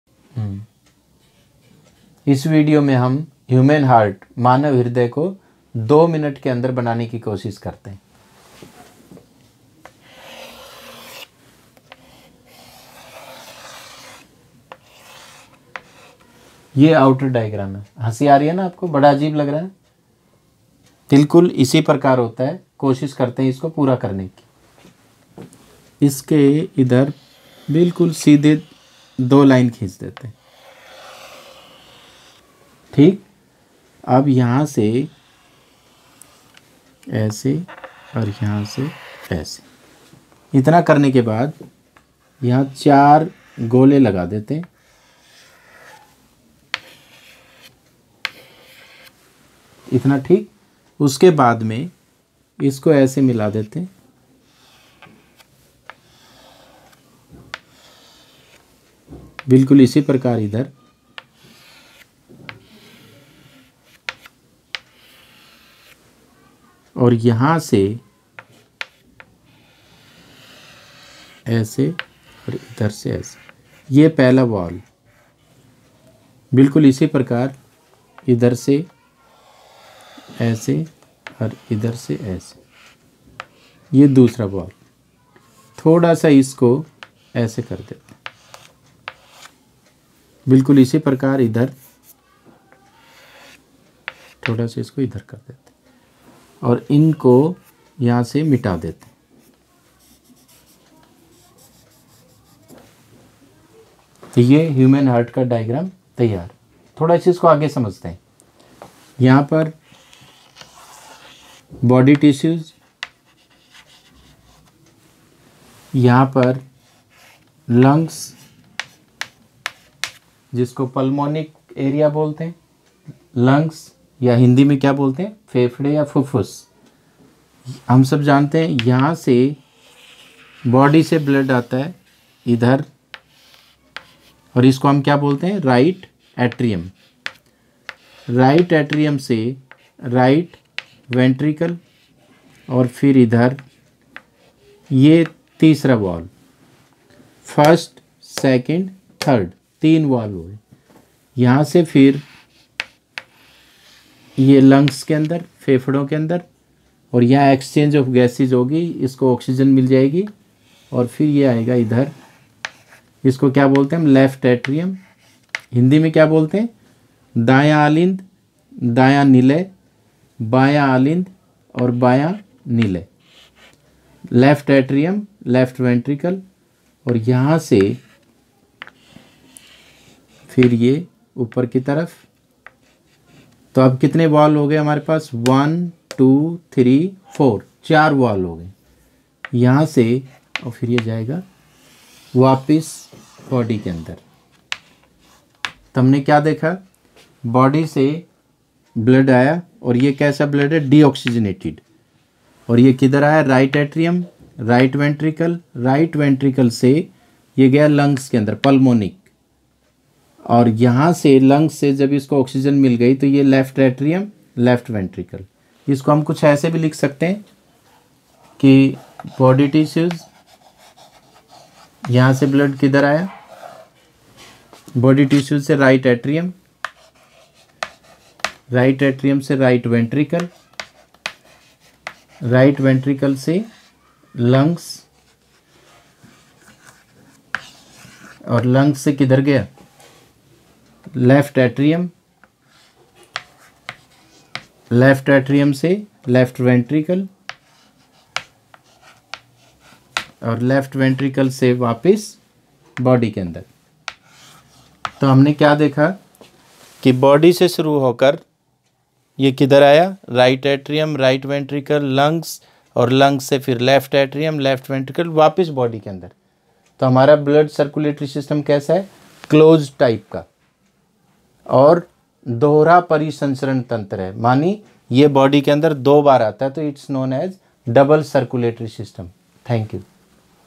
Human Heart diagram in 2 minutes.
Learn how to draw human heart in very easy and fast way